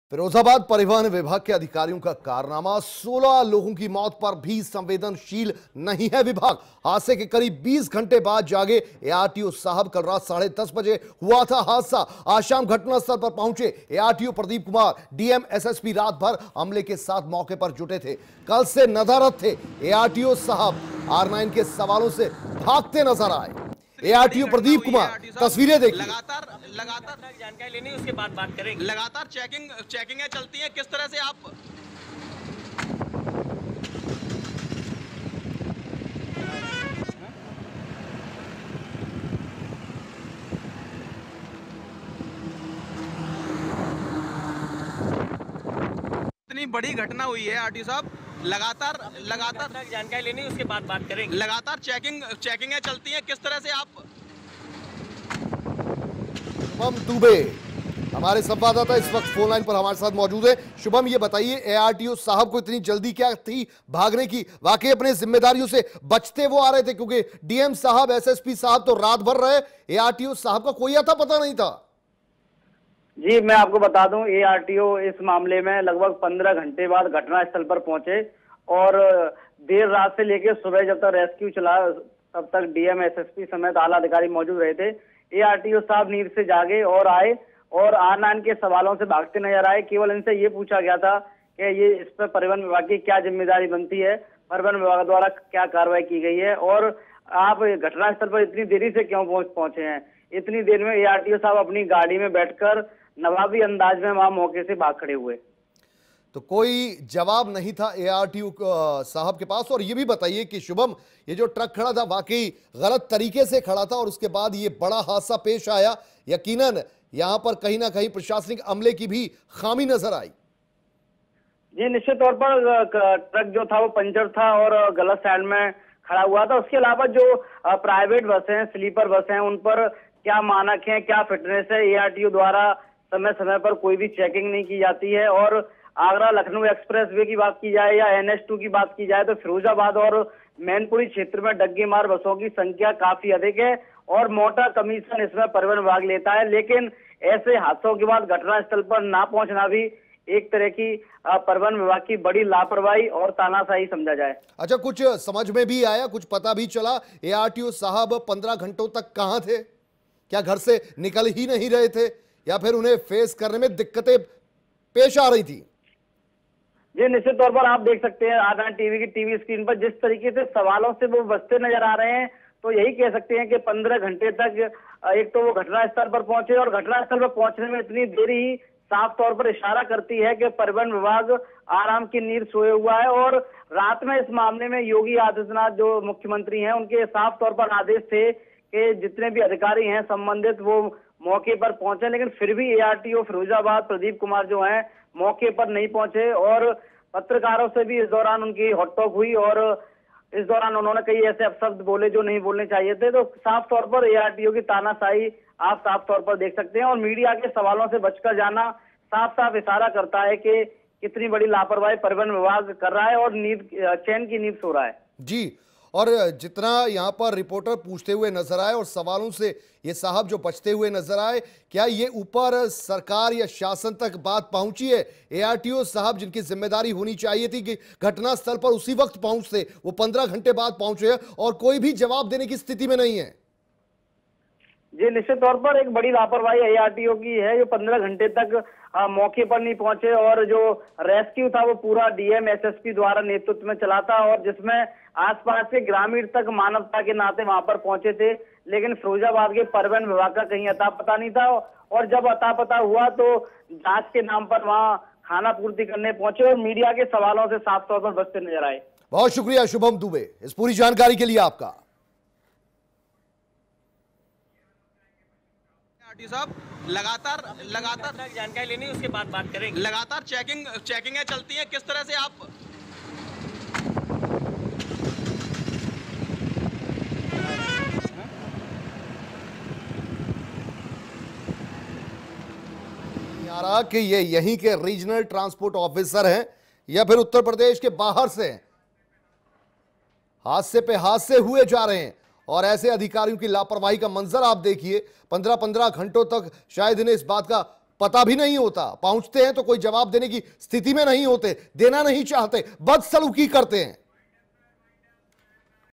16 लोगों की मौत पर भी संवेदनशील नहीं विभाग, घंटों बाद जागे ARTO , R9TV के सवालों से भागते दिखे
फिरोजाबाद परिवहन विभाग के अधिकारियों का कारनामा,
हादसे के करीब 20 घंटे बाद जागे ARTO साहब...